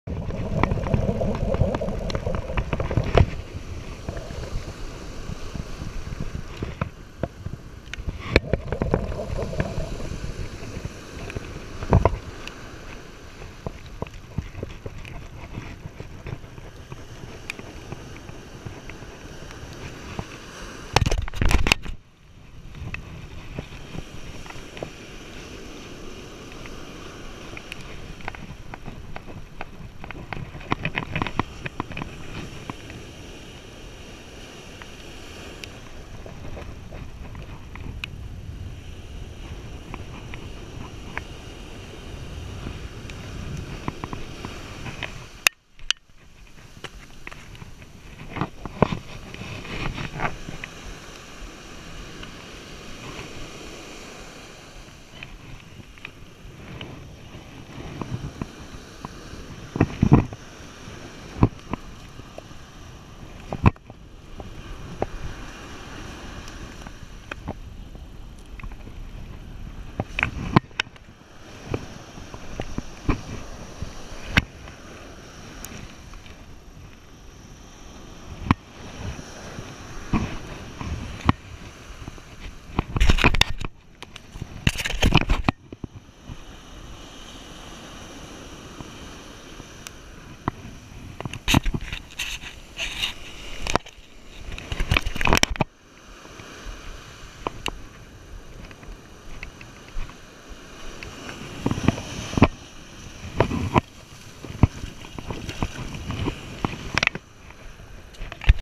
Skate Jones Beach West End 2 Fishing Report June 25, 2015
Skates mixed in with the sand Sharks at night this past week.  No Bass No Blues No Fluke all on chunks herring.  for current reports, tides, moon phase, wind data and weather info for your next outing.